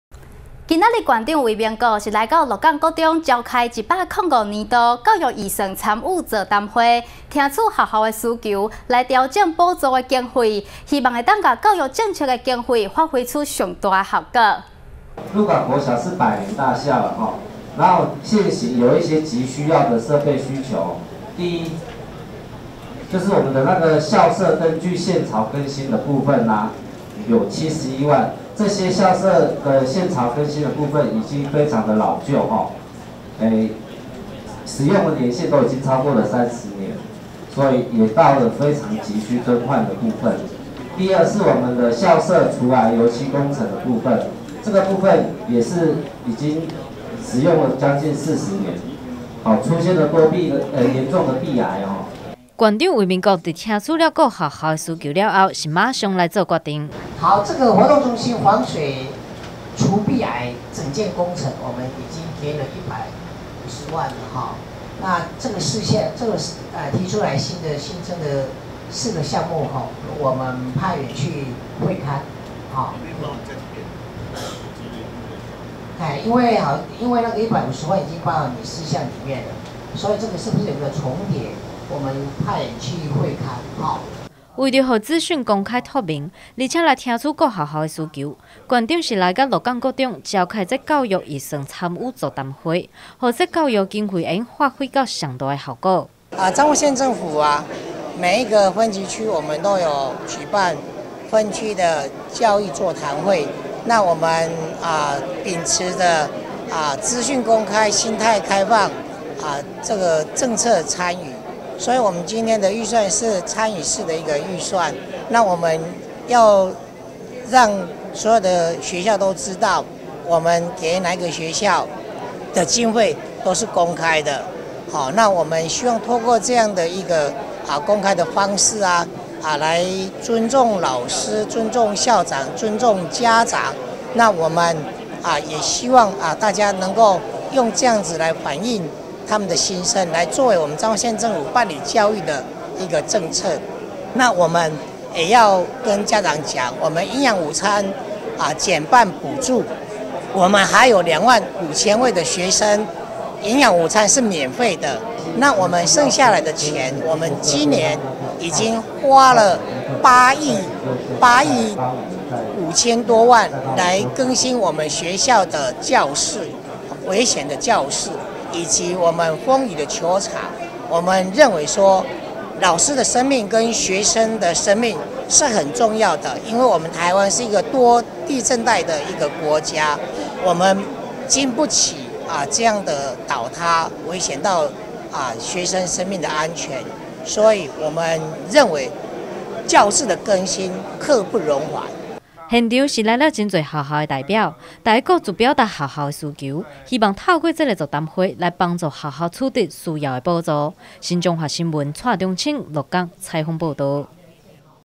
新彰化新聞0627 教育預算座談會 縣長:聽取需求全力補助
今天縣長魏明谷是來到鹿港國中召開105年度教育預算參與座談會，聽取學校的需求來調整補助的經費，希望能把教育政策的經費發揮出最大的效果。